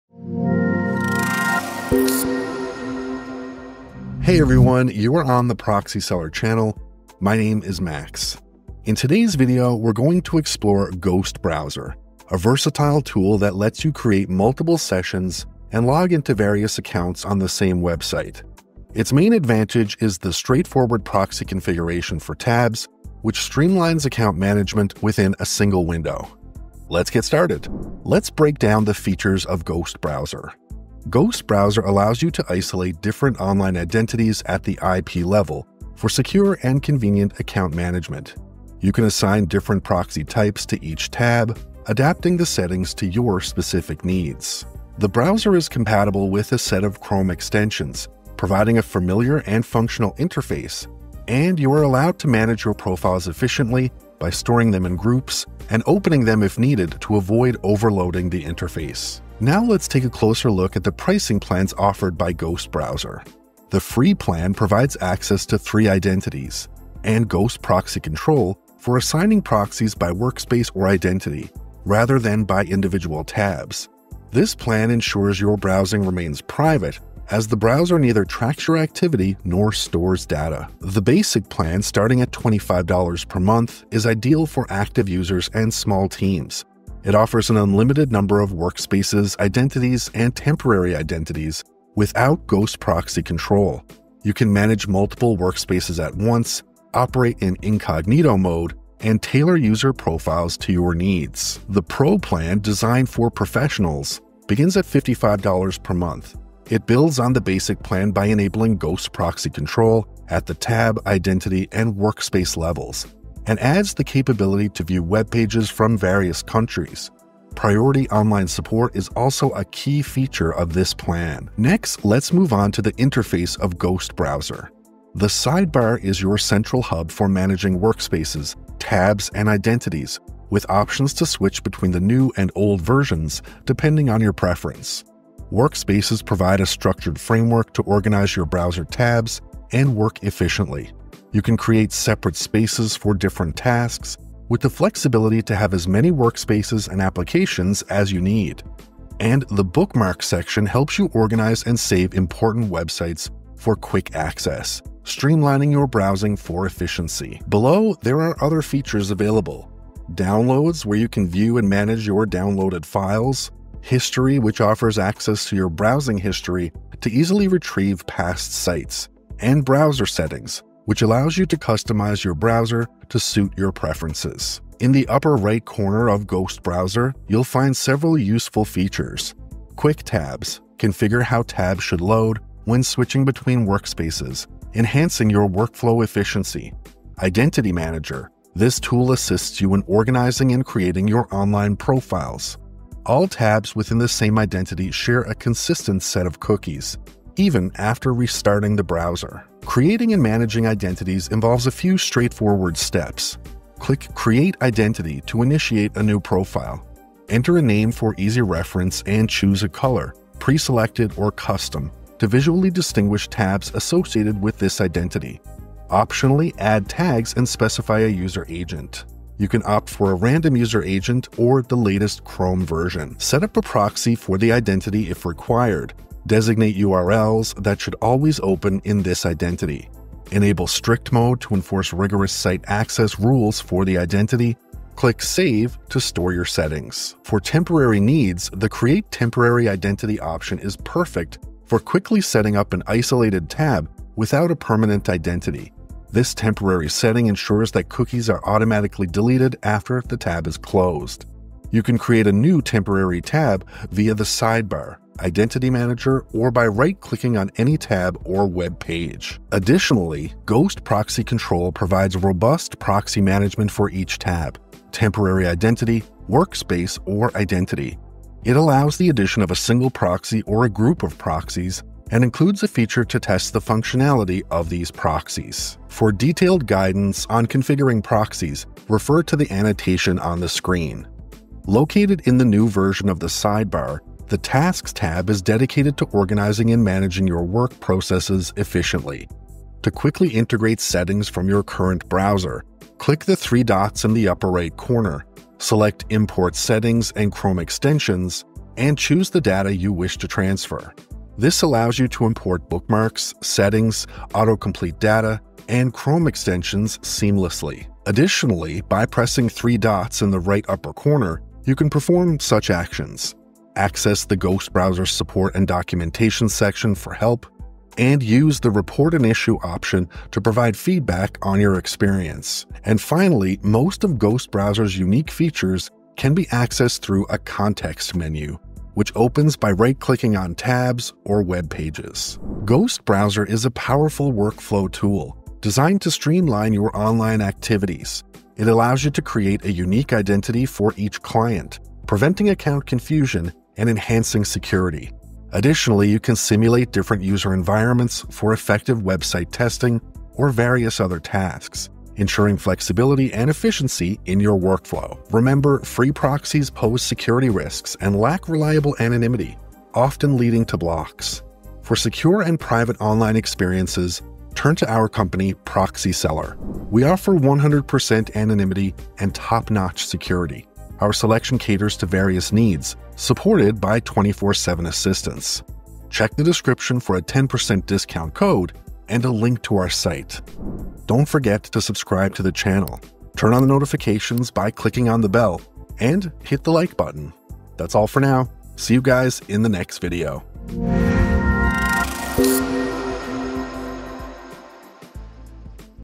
Anti-detect browser review — Ghost Browser
In this video, we'll take a look at the basic functionality, pricing plans, benefits, and interface of the anti-detect Ghost Browser.

00:00 Introduction
00:12 What is Ghost Browser
00:27 Ghost Browser features
00:59 Subscription plans
01:03 Free plan
01:21 Basic plan
01:42 Pro plan
02:01 Interface overview
02:05 Sidebar section
02:15 Workspaces section
02:29 Bookmarks section
02:40 Downloads section
02:44 History section
02:49 Browser settings section
02:59 Quick tabs section
03:07 Identity manager section
03:19 Profile creation
03:57 Temporary profile creation
04:19 Ghost proxy control section
04:41 Tasks section
04:49 Settings import feature
05:08 Support and documentation section
05:33 Conclusion